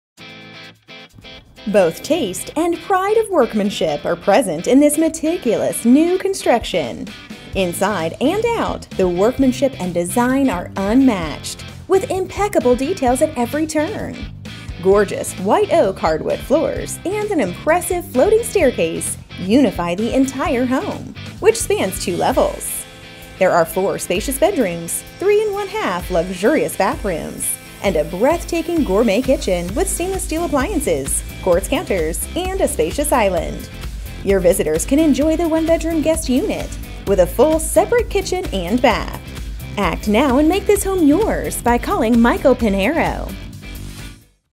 37000 Poplar Street Newark, CA | ColdwellBankerHomes.com
37000 Poplar Street, Newark, CA

Both taste and pride of workmanship are present in this meticulous new construction. Introducing a stunning, newly constructed modern Craftsman home with extraordinary style. Inside and out, the workmanship and design are unmatched with impeccable details at every turn. Gorgeous white oak hardwood f